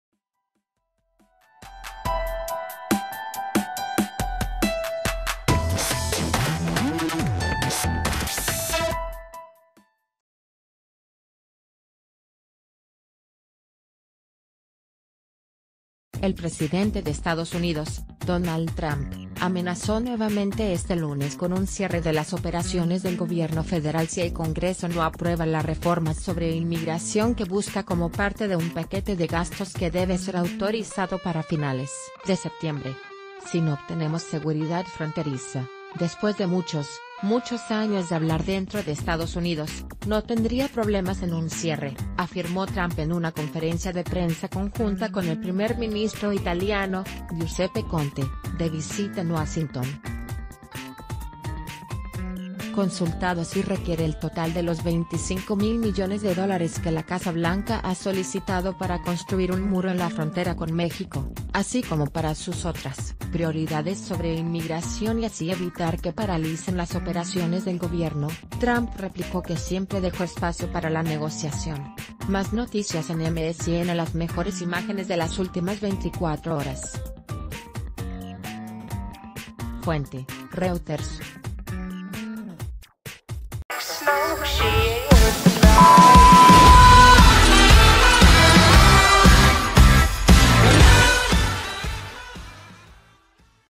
Trump reitera su amenaza de cerrar gobierno si demócratas no apoyan el muro
El presidente de Estados Unidos, Donald Trump, amenazó nuevamente este lunes con un cierre de las operaciones del Gobierno federal si el Congreso no aprueba las reformas sobre inmigración que busca como parte de un paquete de gastos que debe ser autorizado para finales de septiembre."Si no obtenemos...